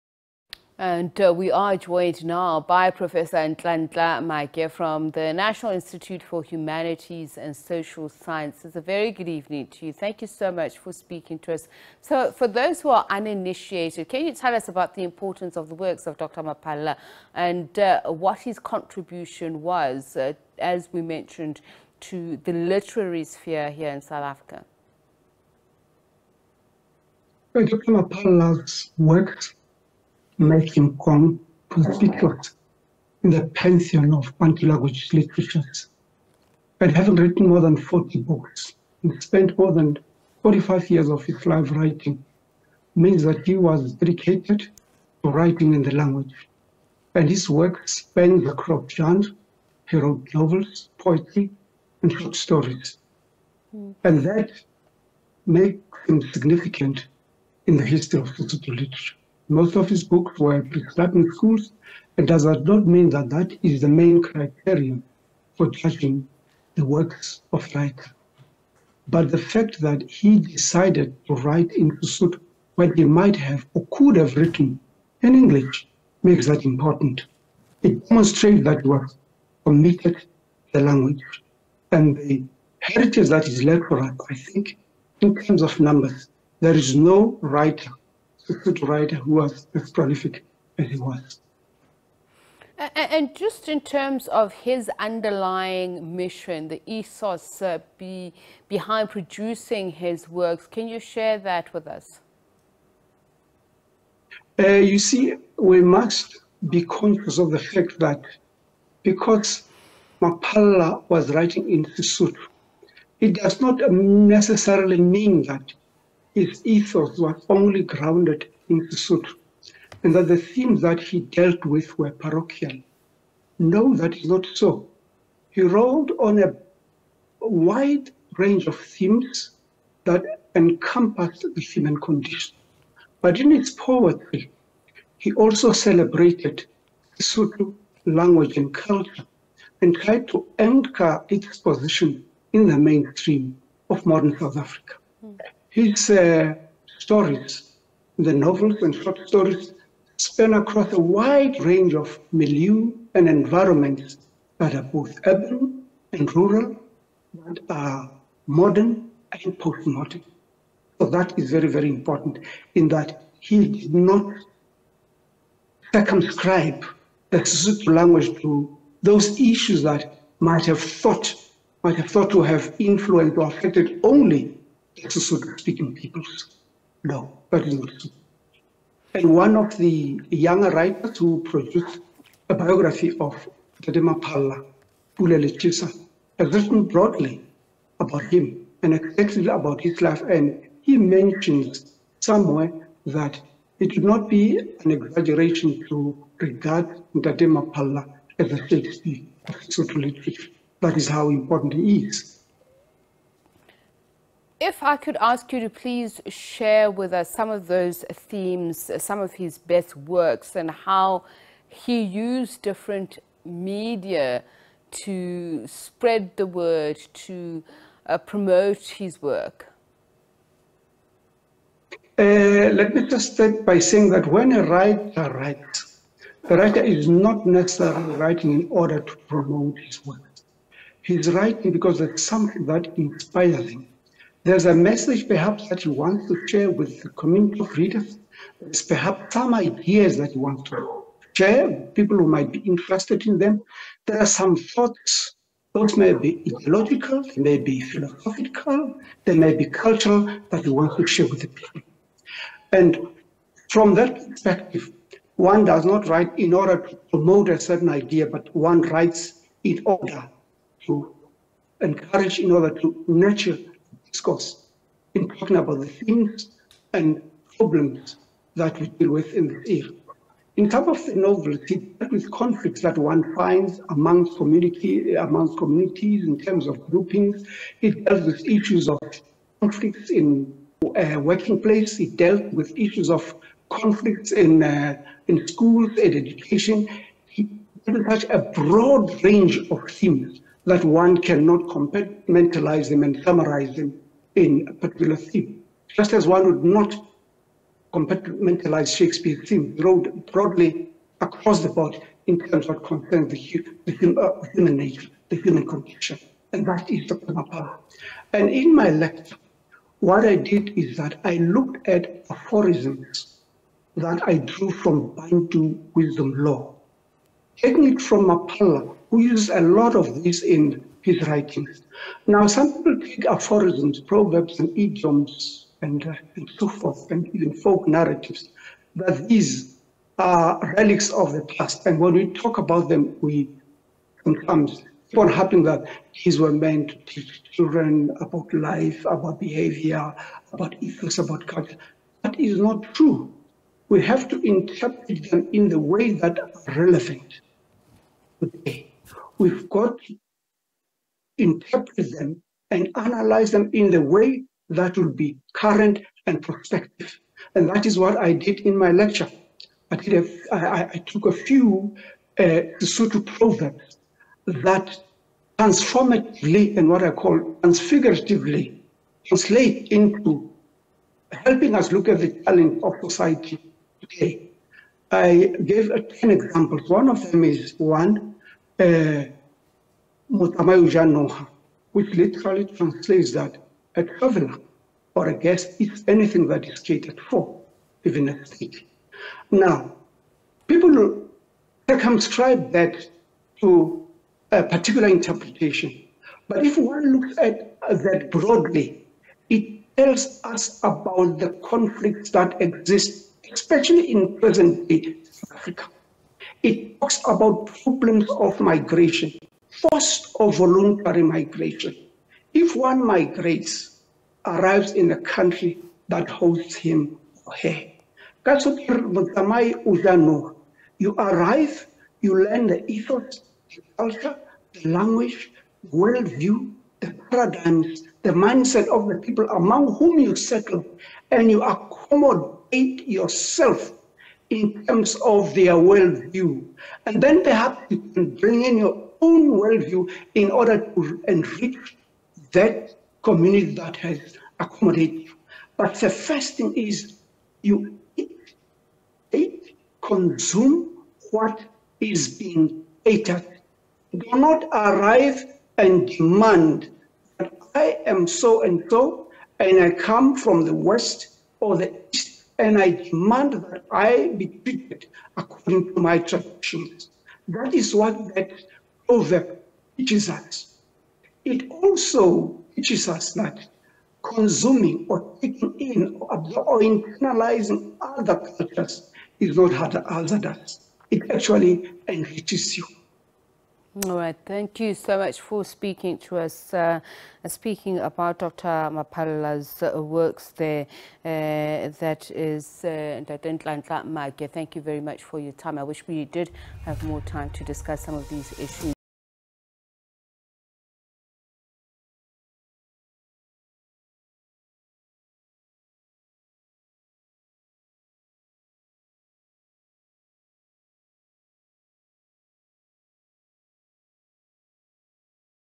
Dr KPD Maphalla Memorial Lecture: Prof Nhlanhla Maake
Joining us via Zoom is Professor Nhlanhla Maake from the National Institute for the Humanities and Social Sciences.